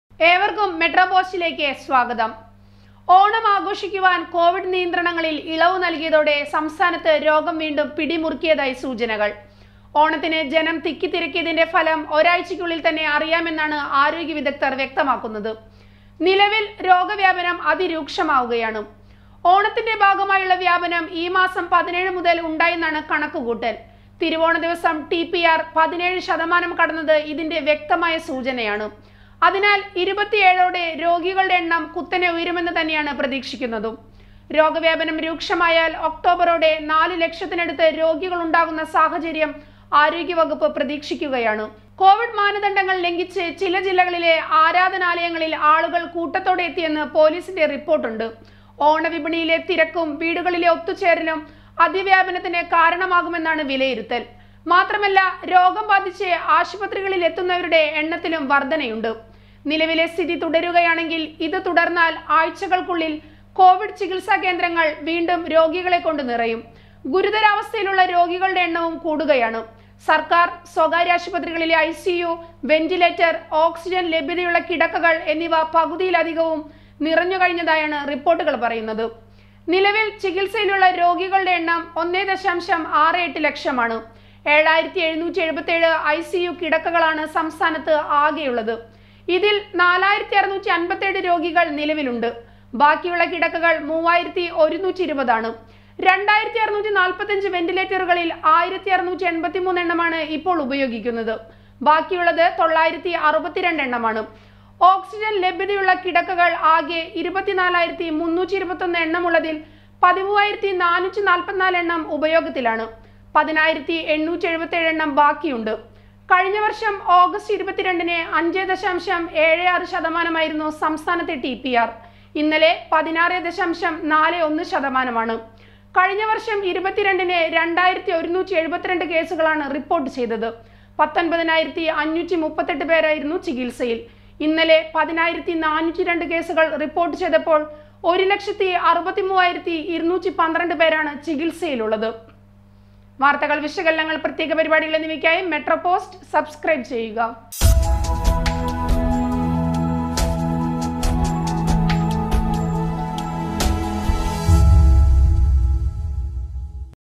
ഓണം കഴിയുന്നതോടെ വീട്ടിലിരിക്കാൻ ഒരുങ്ങിക്കോളൂ: ആശുപത്രികൾ പകുതിയിലധികം നിറഞ്ഞു കഴിഞ്ഞു | METRO POST
ഓണം കഴിയുന്നതോടെ വീട്ടിലിരിക്കാൻ ഒരുങ്ങിക്കോളൂ:
ആശുപത്രികൾ പകുതിയിലധികം നിറഞ്ഞു കഴിഞ്ഞു